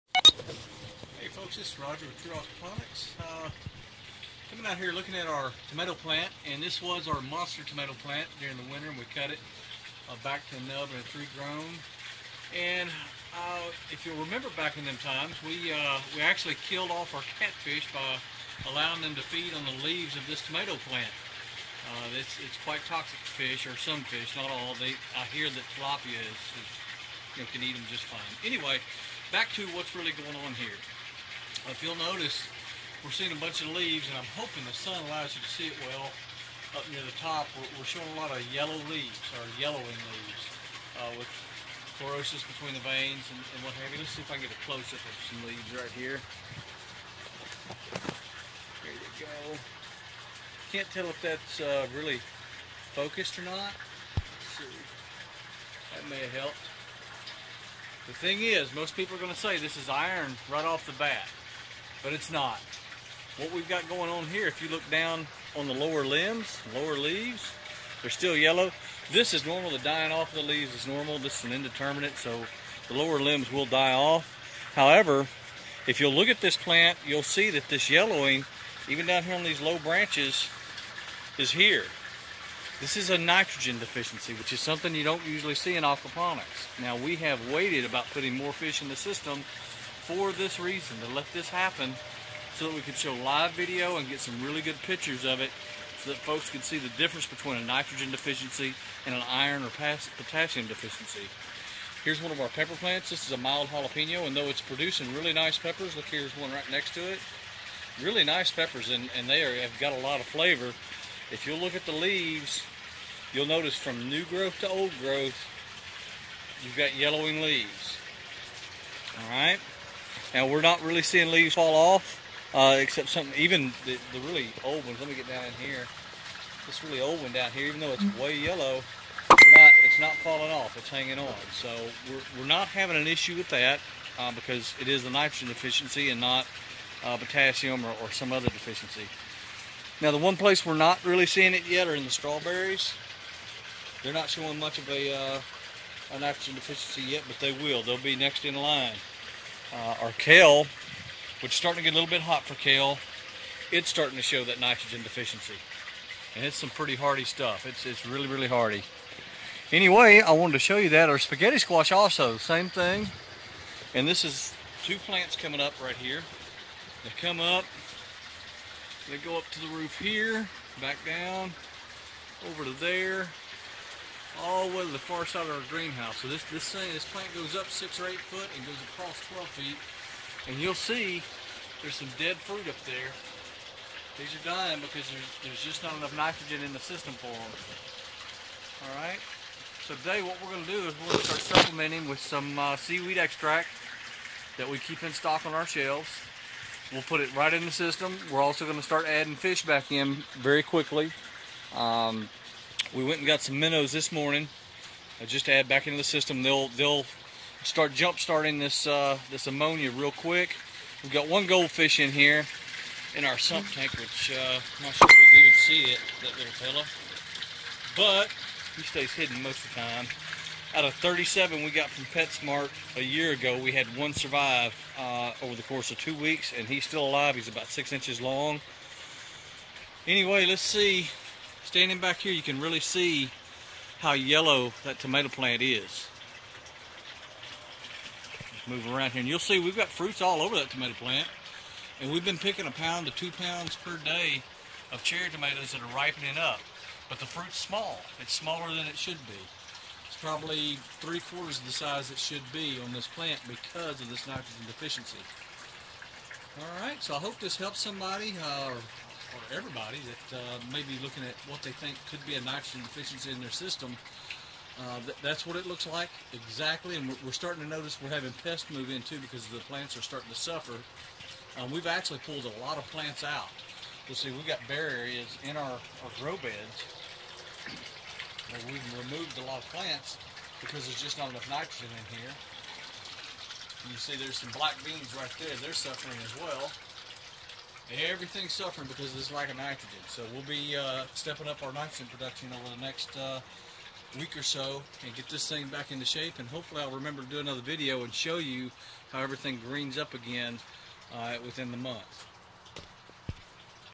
What a nitrogen deficiecy looks like.
Back in the spring of 2015 we trimmed the tomato plant that is in this video, and some of it fell into the fish tank. The catfish loved the leaves so we let them take their fill. This was a mistake as it is toxic to them. We lost 80 channel cats that were just over a pound each in one day. We decided to keep the system low on fish for awhile so that we could show the one deficiency that hardly is ever seen in aquaponics.